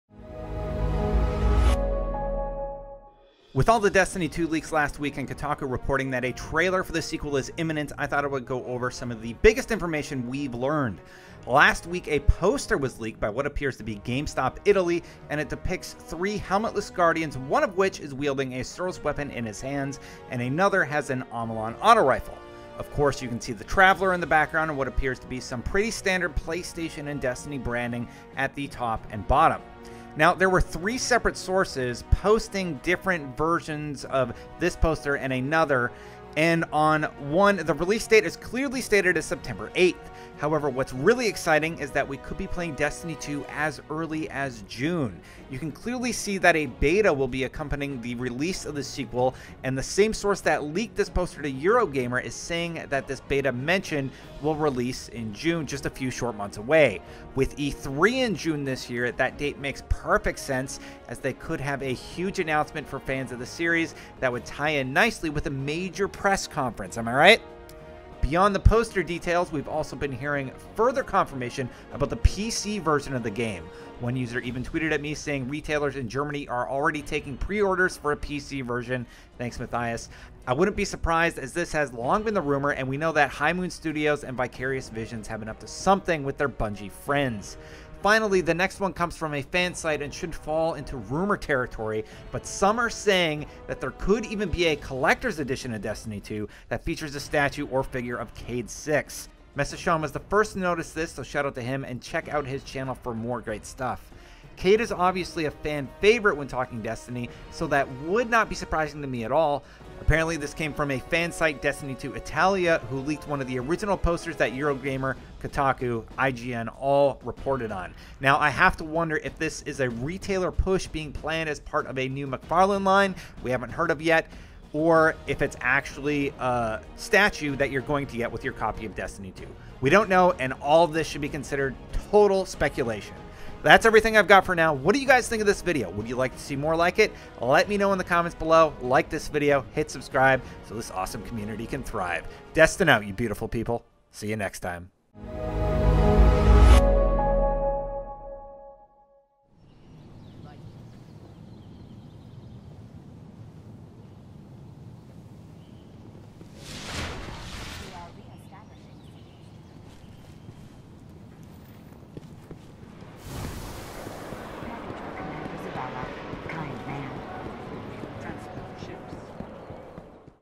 Destiny 2 News Update! Beta Incoming, September Release, PC, and Statue Editions!
Destiny 2 posters have leaked indicating a release date, beta date, and rumors are flying about PC pre-orders and Cayde statues!

Tuesdays / Thursdays at 8pm